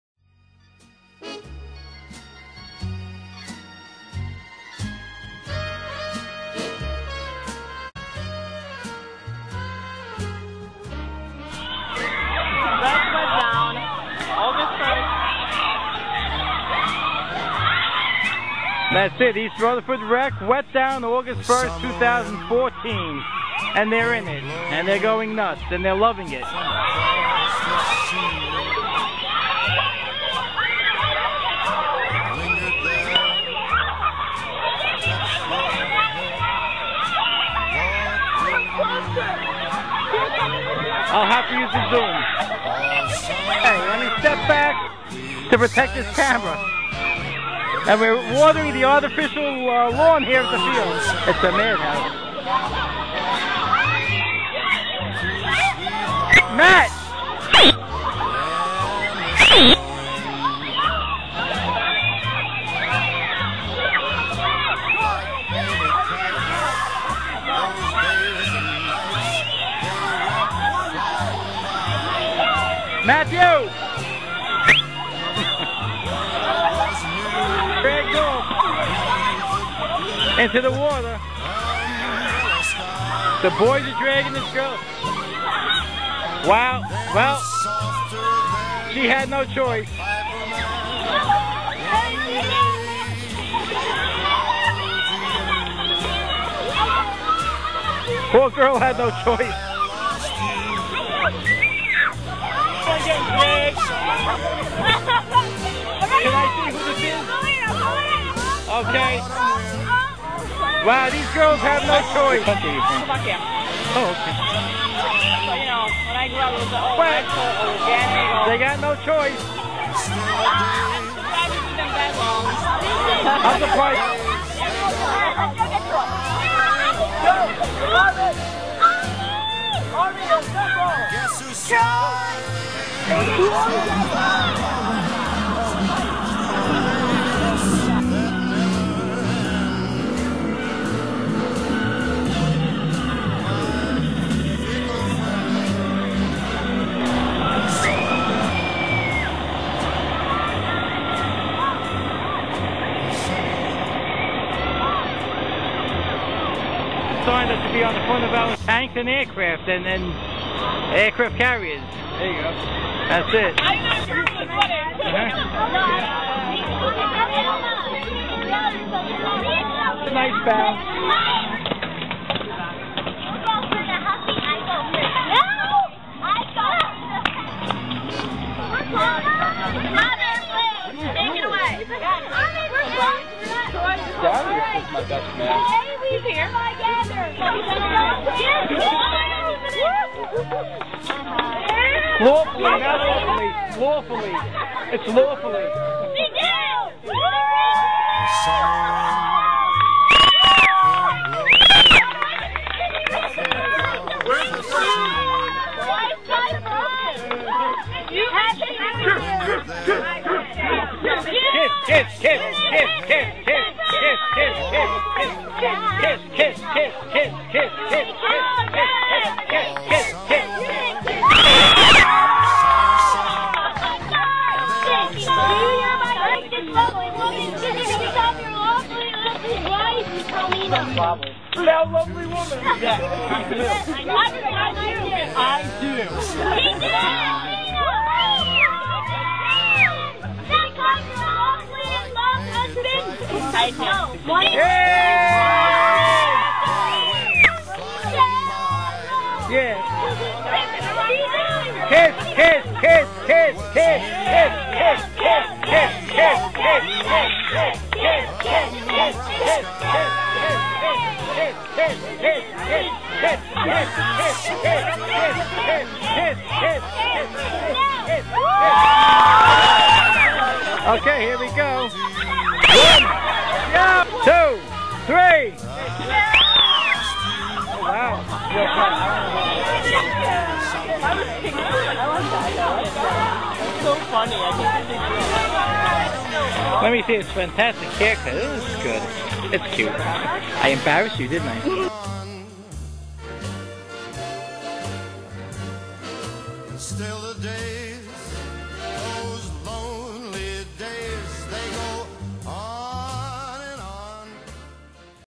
East Rutherford Rec 2014 by Nick Belmonte of gourmetreviews net
East Rutherford, NJ probably the best little town in the U.S.A.. Everyone has a big heart and this town does so much for the kids. Thank you Jogn Hansen, Mayor Cassella, the police & fire department and the town council for making this possible. GOD bless!